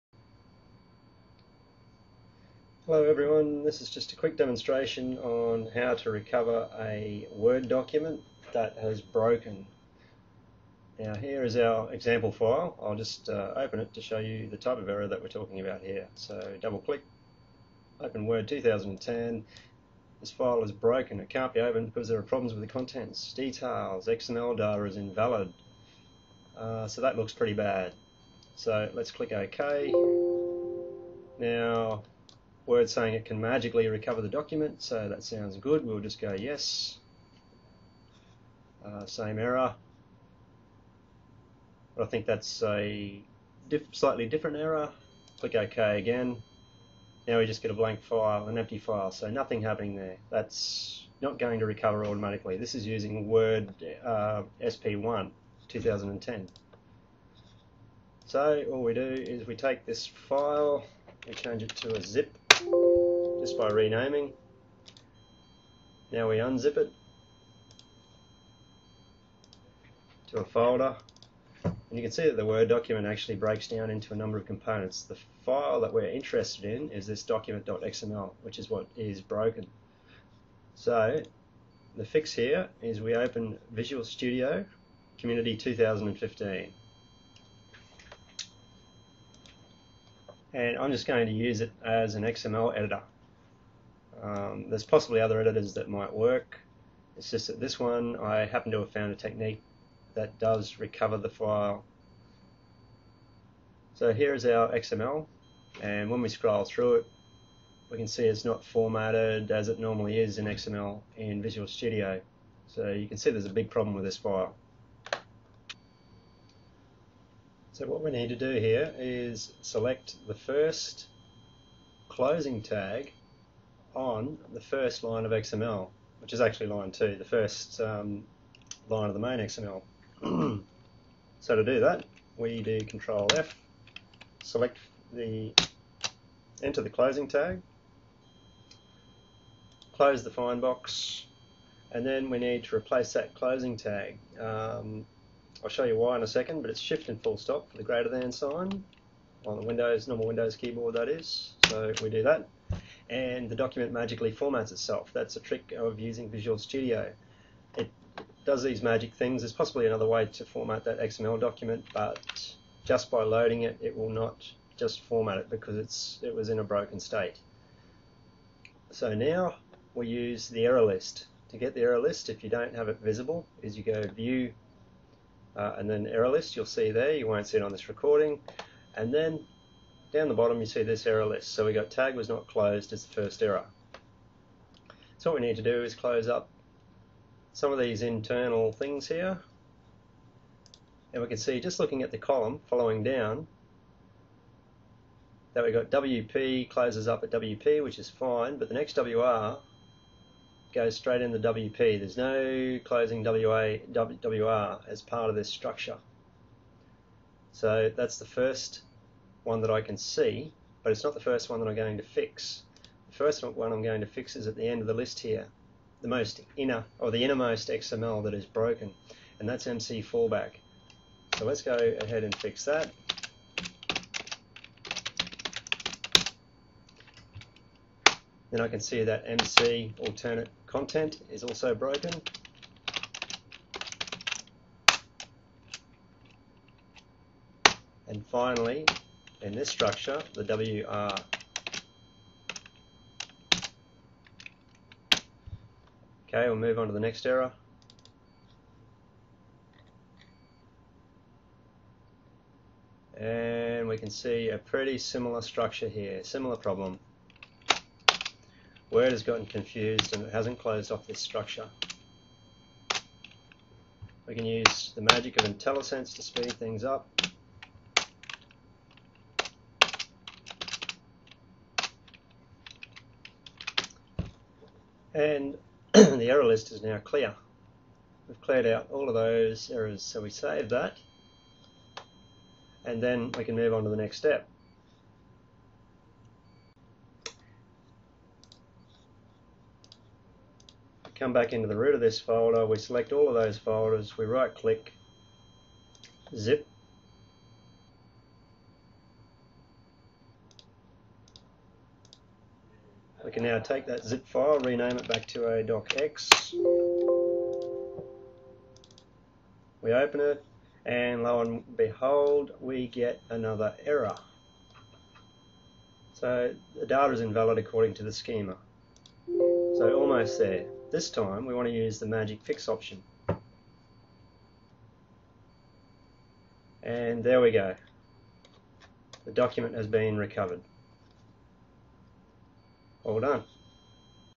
Recover a CORRUPT Word Document Yourself for FREE!
How to recover a corrupt/damaged Microsoft Word document from errors like:

The file cannot be opened because there are problems with the contents.

Details:
The XML data is invalid according to the schema.

Location: Part: /word/document.xml, Line: 2, Column: x

This worked where automatic recovery failed, and none of the online methods worked for me, this was my own diagnosis and repair solution.  Other versions of Visual Studio will likely work too, such as the express and commercial versions.  Express and Community are free.

As a side-note, instead of replacing the closing tag as per my video, you can simply go to edit-advanced-Format Document to achieve the same thing a little more easily.  Thanks to Bryce Geoghegan for this tip!

If you can't extract the docx there doesn't tend to be any coming back from this unfortunately, there's nothing to work with.  Try 'restore previous version' in Windows Explorer, though most people won't have that setup properly (hint: I don't use that, it's a last resort option if you don't have any kind of backup for your file, read on).

Prevention: sync your document with Google Docs, even if it is a native Word file (and not a Google docs file), you can always go back and retrieve previous versions, make sure that feature is turned on.  Always have some kind of backup for your precious files, Google Docs hasn't let me down yet, just make sure it doesn't silently crash making your files unprotected!